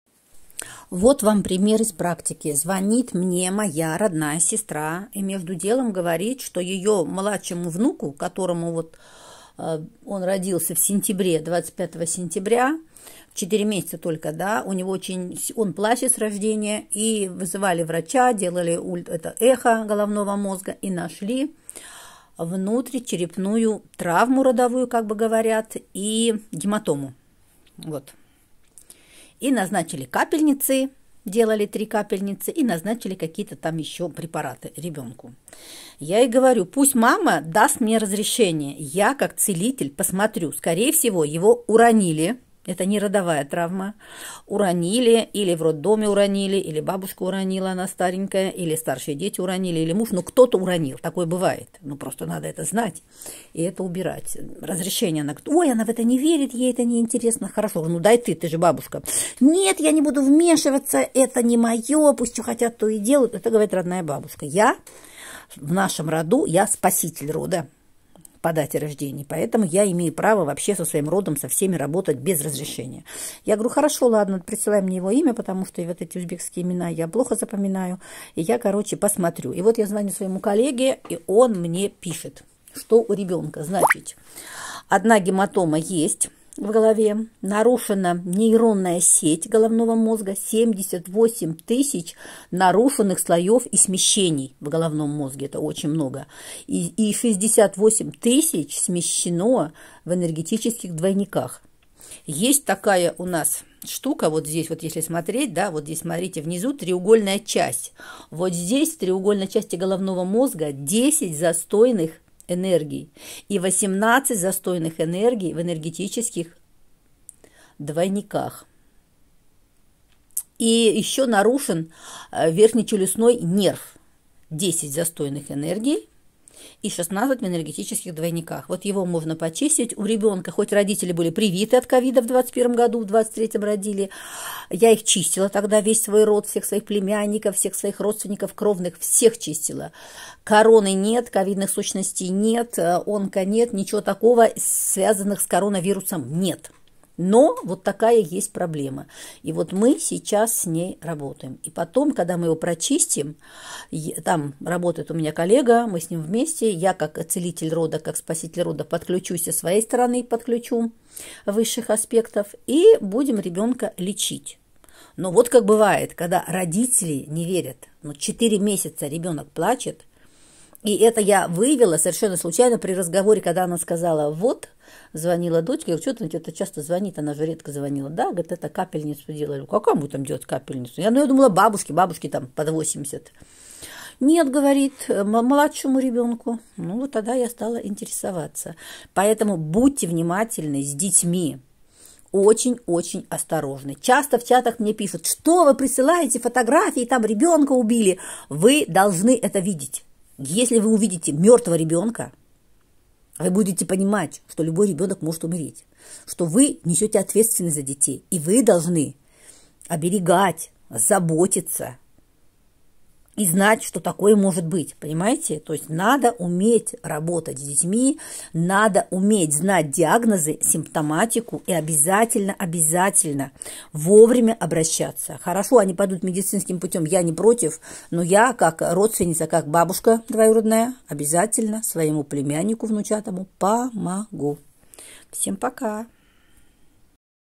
Родовая травма/ внутричерепная гематома новорожденного
Я обычно не вмешиваюсь без разрешения. но это мой родной внучатый племянник.